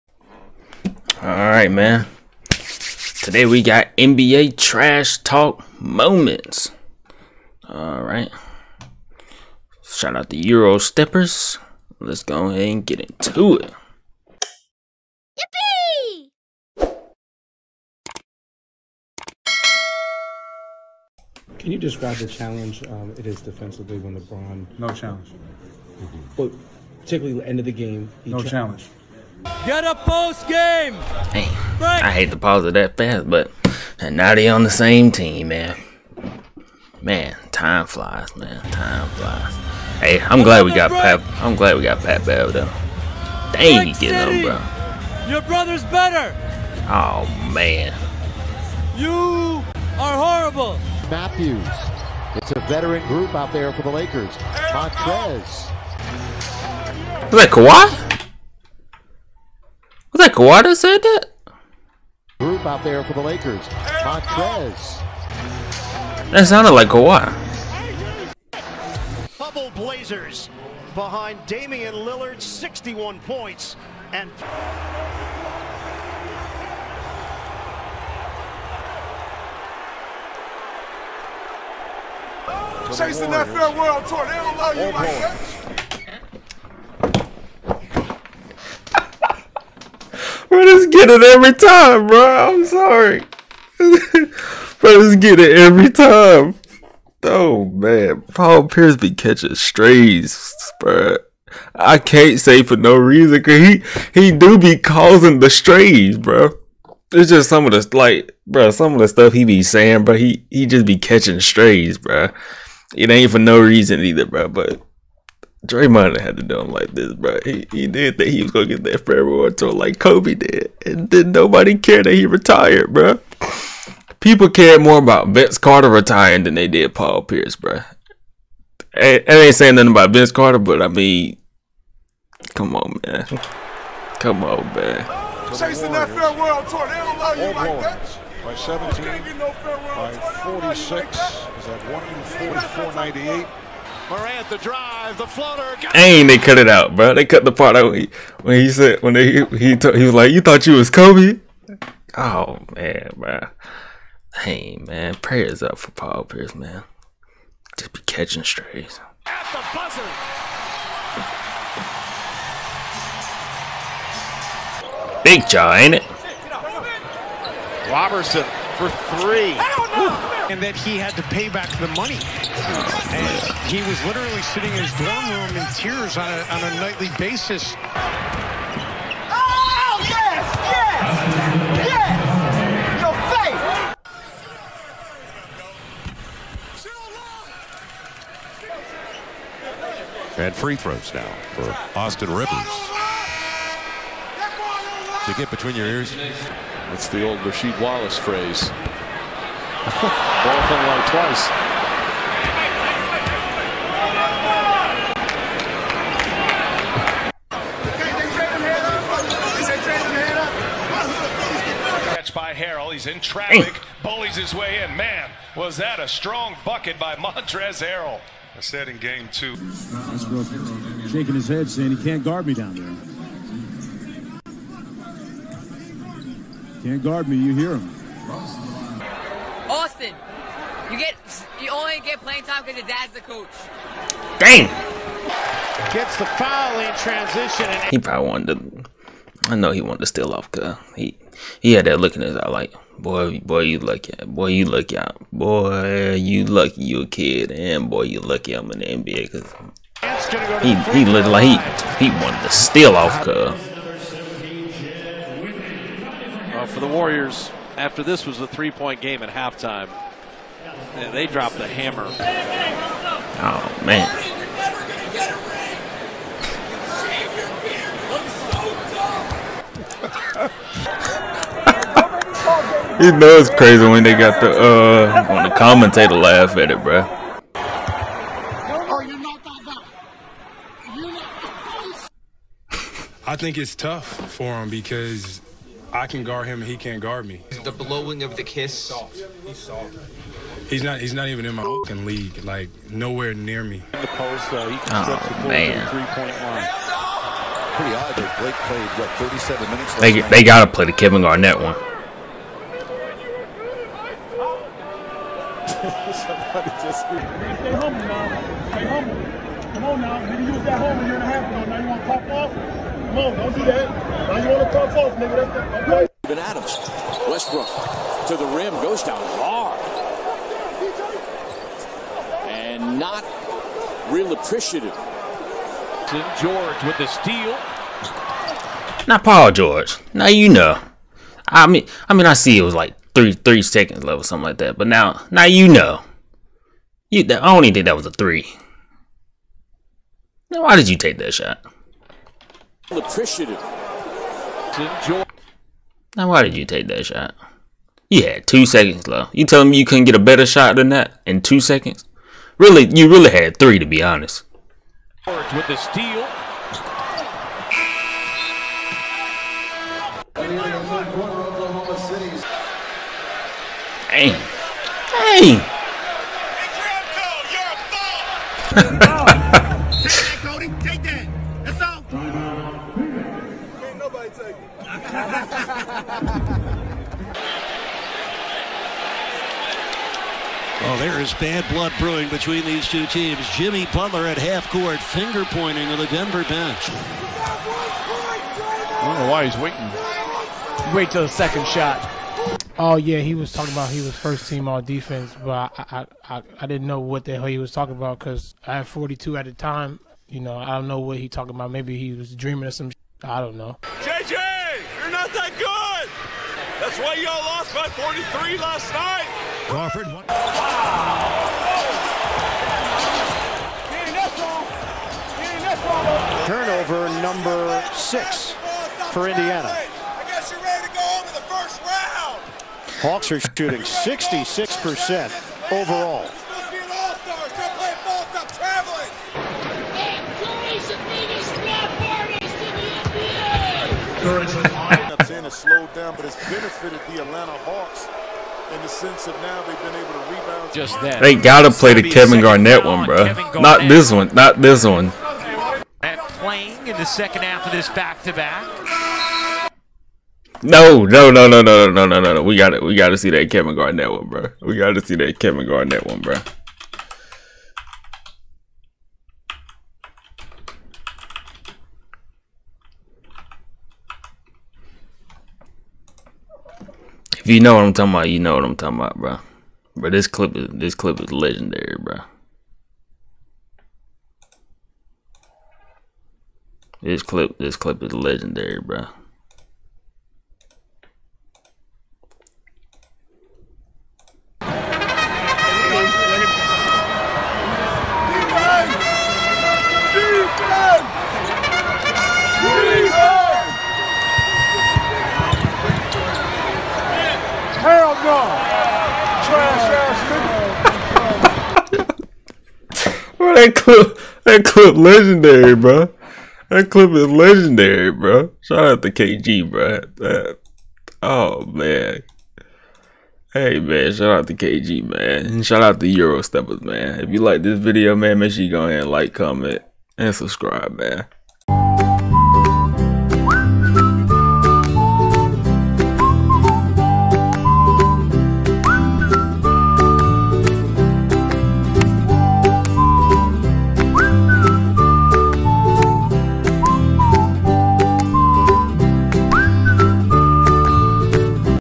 DatBoyKB Reacts to "NBA TRASH TALK MOMENTS"!!
Ayeeee it´s ya boy KB with another video. I´m still kinda new with the editing so spare me!!
              L.L.P.G                                                                             ¨Live Life Playing Games¨